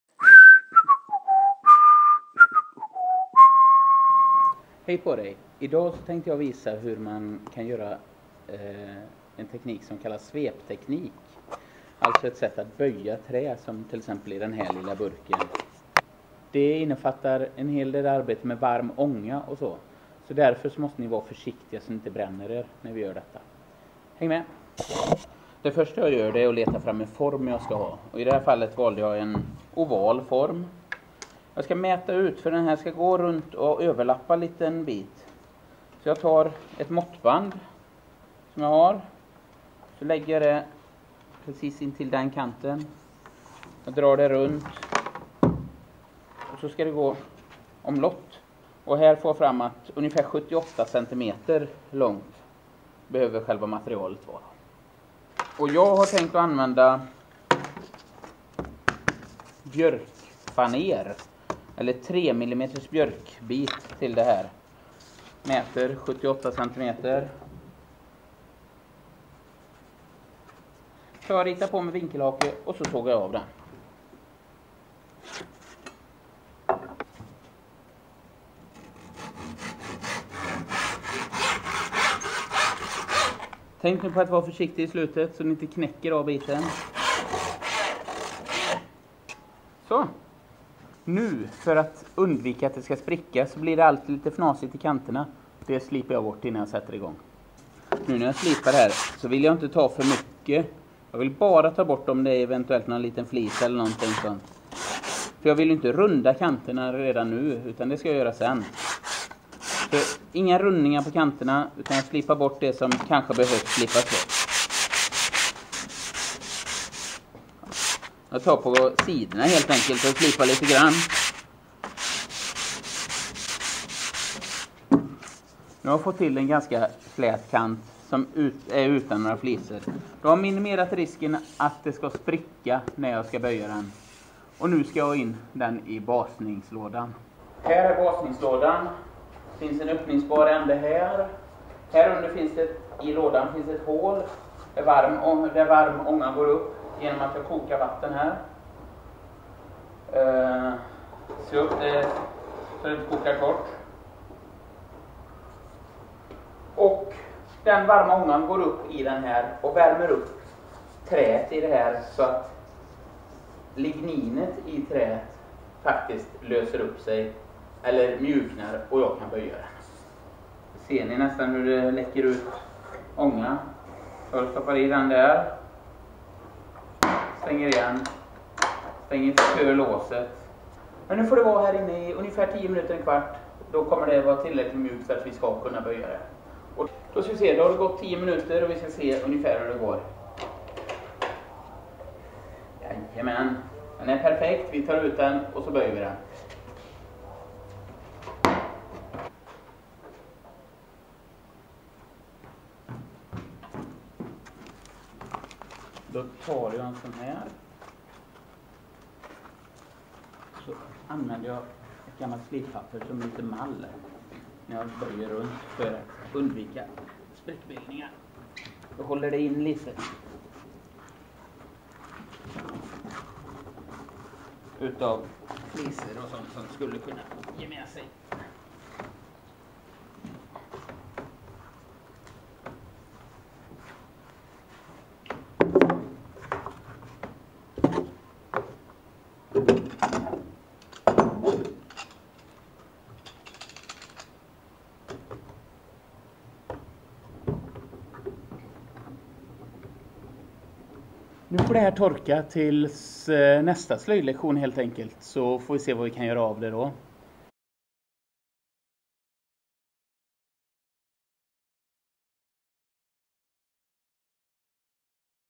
Svepteknik 1 - material, basa och böja
Den här videon handlar om Svepteknik och det första i att göra en svepask, svepfat, eller något annat som innefattar svepteknik.
Här kommer jag visa hur du tar fram material, hur du basar det, och hur du böjer det runt din mall eller form.